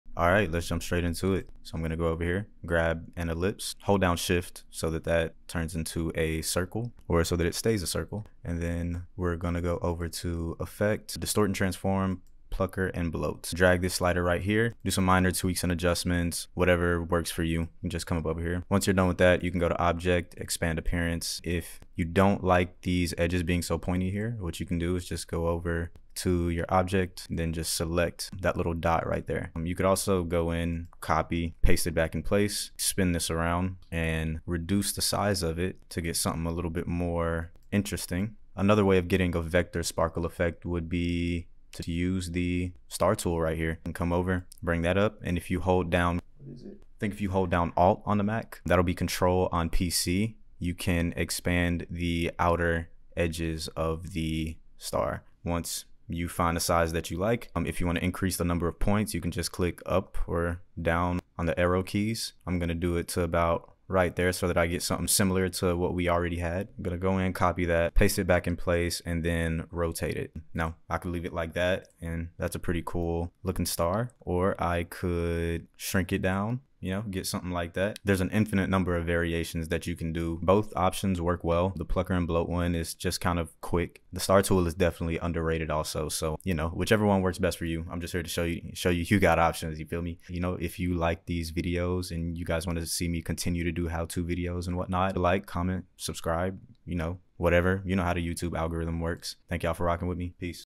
How to Create Vector Sparkles in Adobe Illustrator
Learn how to create some vector sparkle effects in Adobe Illustrator! In this tutorial, we'll explore two different methods: using the distort and transform tool to create a bloat effect, and using the star tool to create a dynamic star shape.

🥳 Snag Some Free Graphic Design Resources!